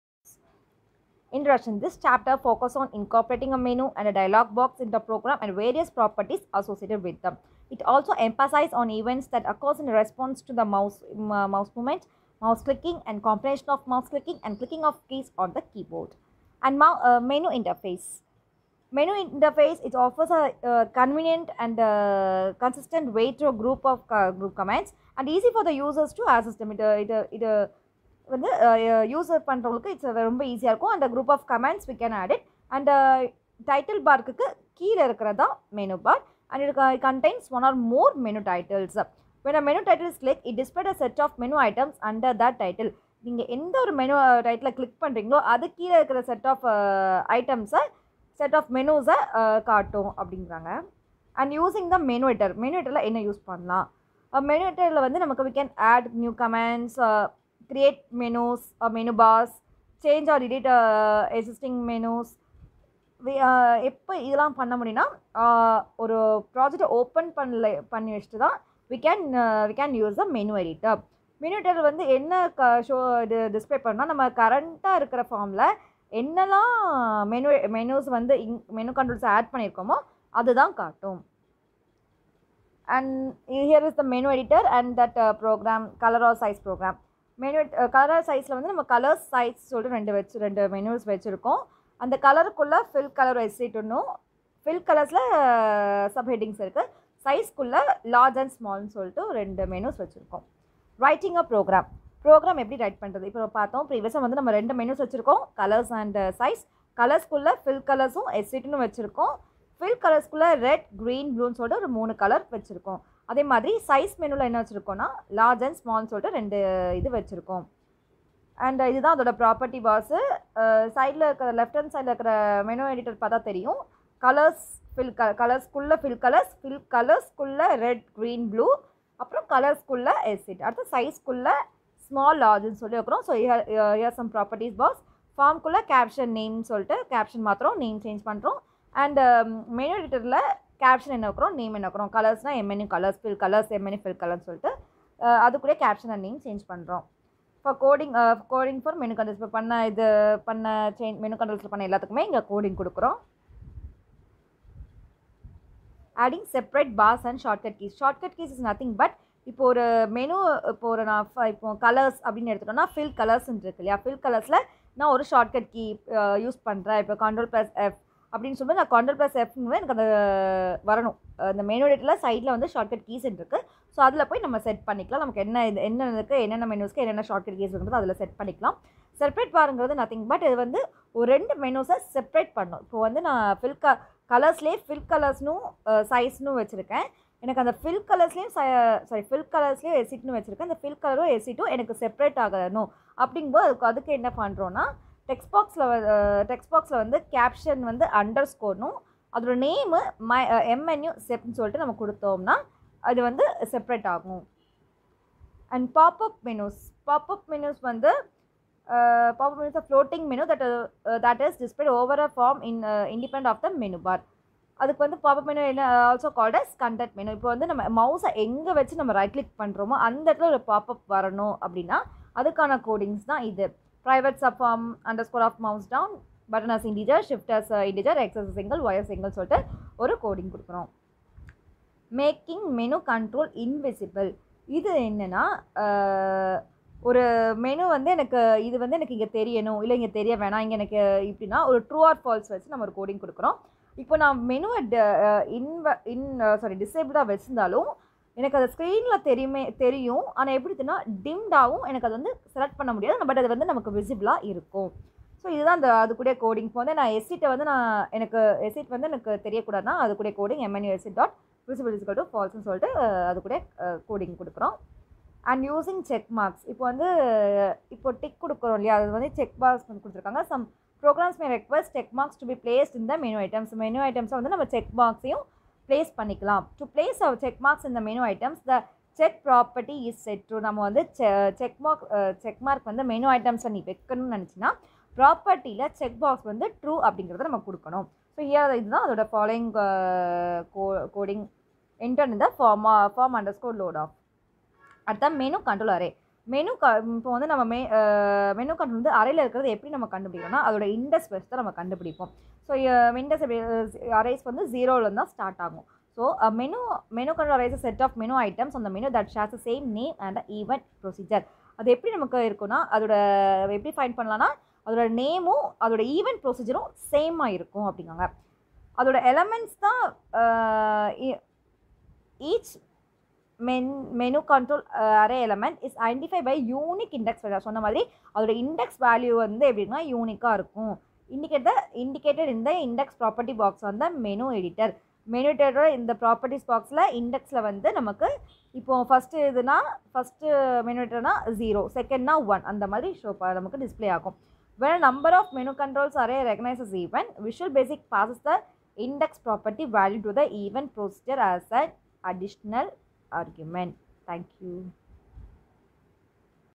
Introduction to Menus , Mouse Events and Dialog Boxes | Visual Basic | Educatcomp | Tamil
Hy this is our dept student created a ppt on particular topic in Visual basic. Let's Learn together to bring the future bright.